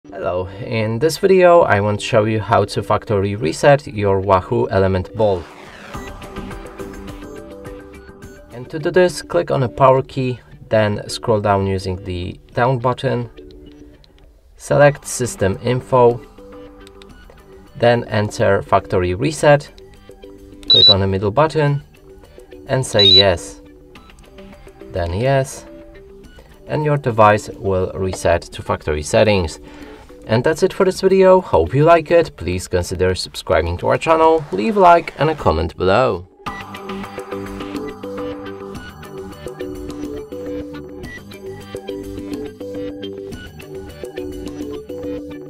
How to Factory Reset Wahoo Element Bolt: The Ultimate Guide - Reset Wahoo Element Bolt in Seconds
Are you having issues with your Wahoo Element Bolt? Is it unresponsive or not functioning properly? In this video, we will show you how to factory reset your Wahoo Element Bolt in just 5 simple steps. By the end of this tutorial, your Wahoo Element Bolt will be as good as new!

How to Perform a Factory Reset on a WAHOO Elemnt Bolt GPS V2?
How to restore default settings on a WAHOO Elemnt Bolt GPS V2?